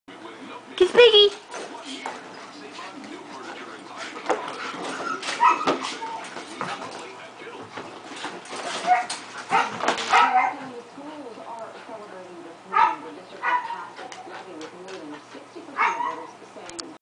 Playing with piggy
husky playing with a 3 legged rubber pig